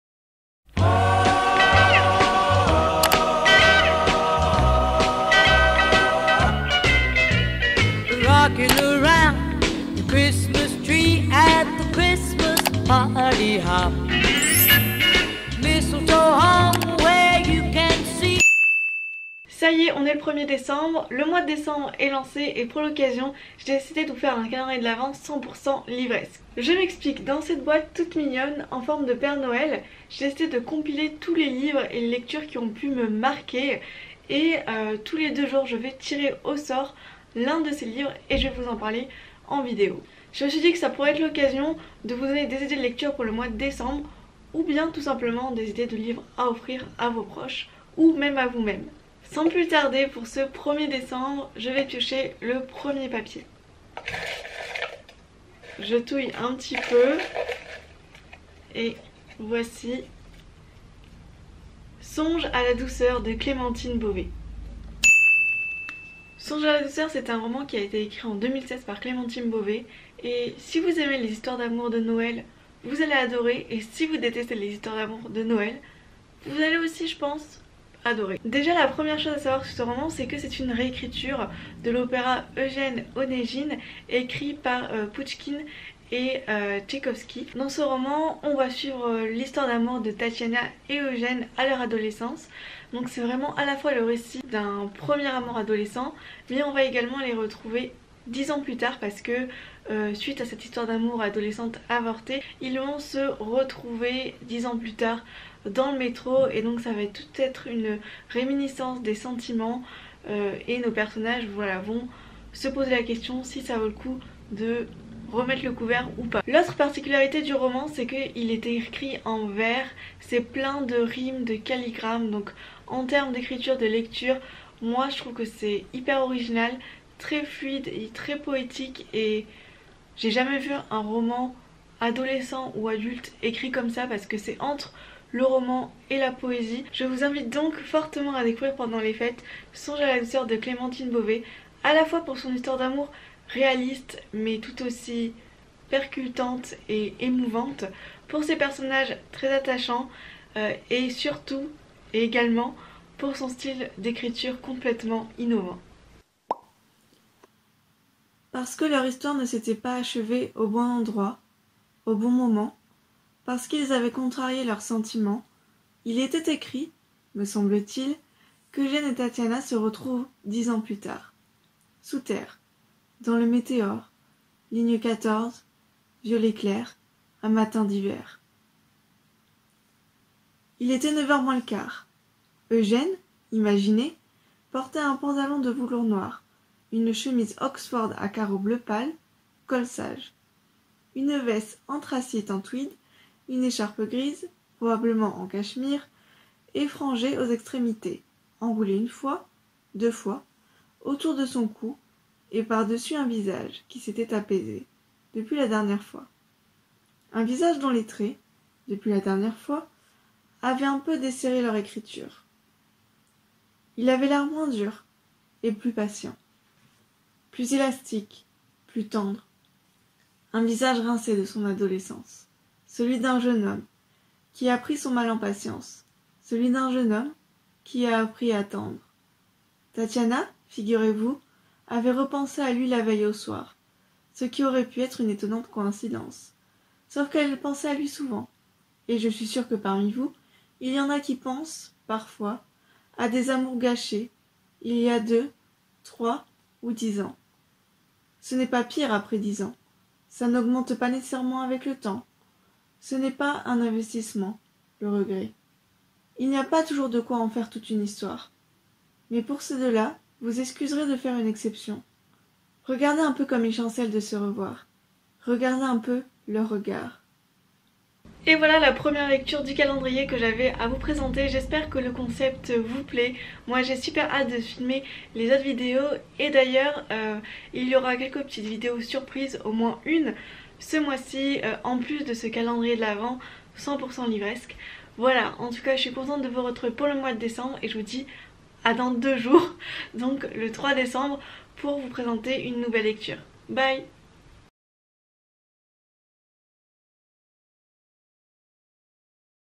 calendrier de l'avent surprise ! 🎅 (jour1) | 12 livres avant noël
↓Bienvenue dans mon journal vidéo 🤍 bienvenue dans mon petit univers et mon aesthetic↓

Jour 1 de ce calendrier de l'avent 100% livres. Chaque jour, je pioche au hasard dans une boîte contenant mes livres préférés et je vous en parle.

🎥 : Sony A5000